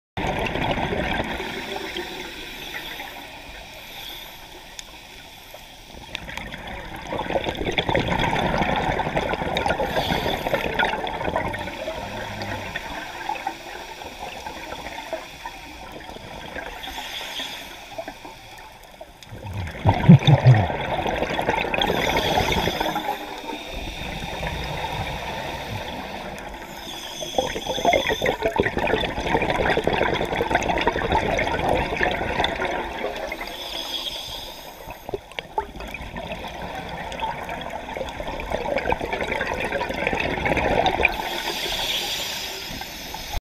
Egg yolk does not burst at 30m underwater
Due to the pressure, this egg yolk is pretty resistent